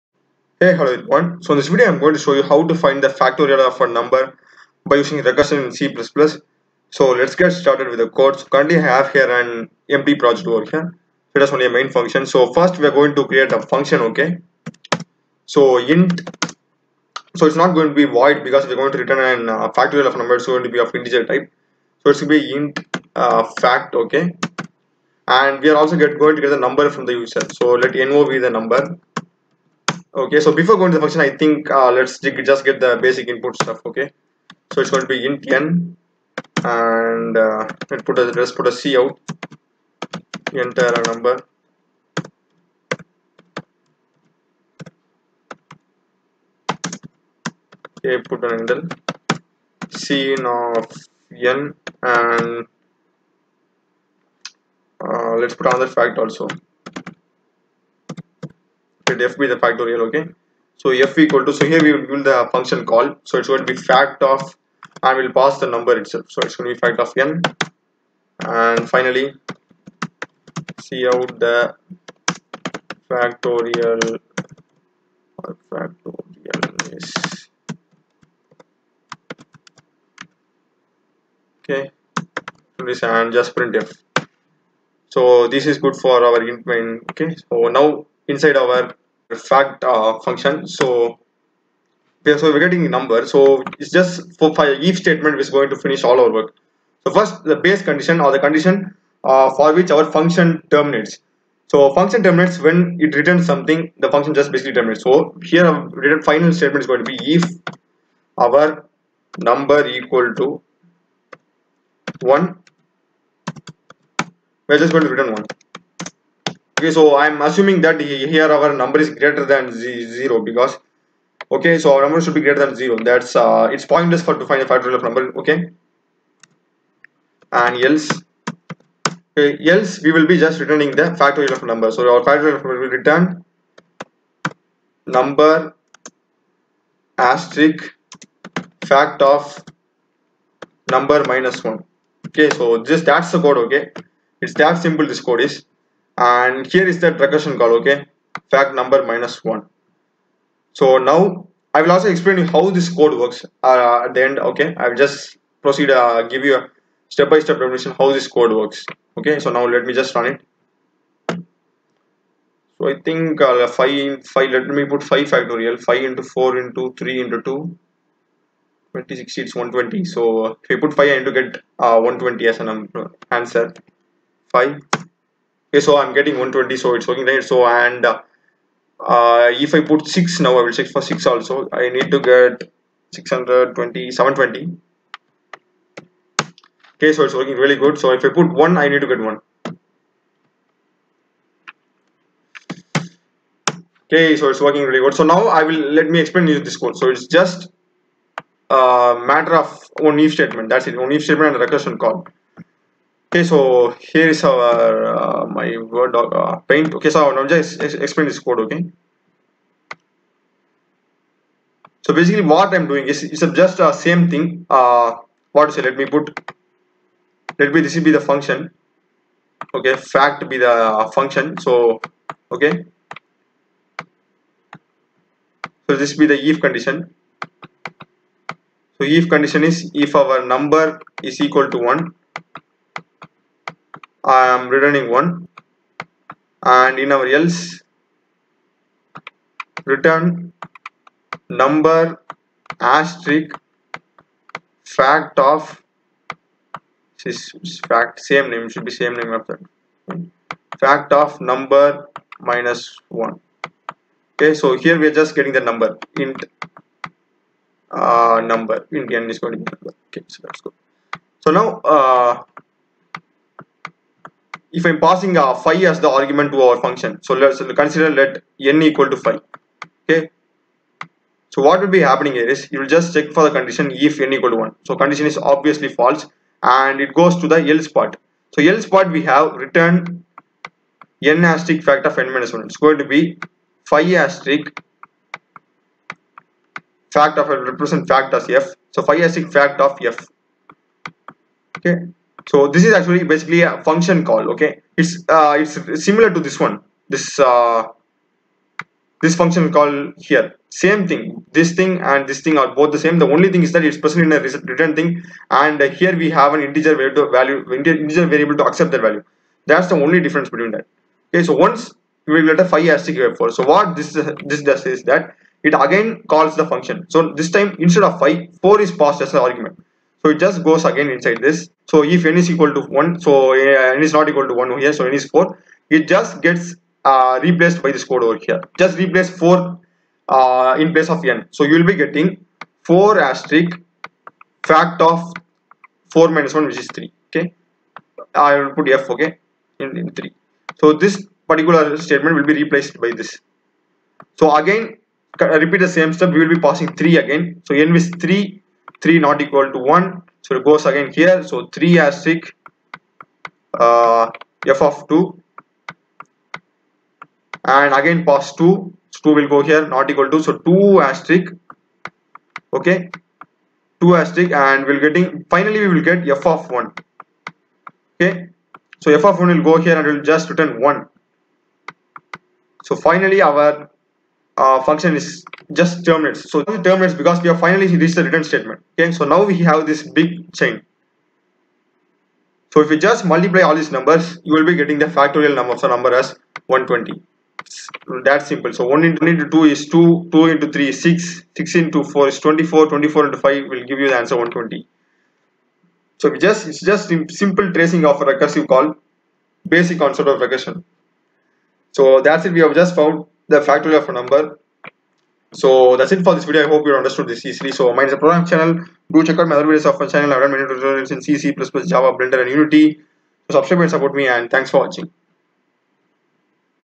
C++ program to find factorial of a number using recursion tutorial
C++ program to find factorial of a number using recursion is shown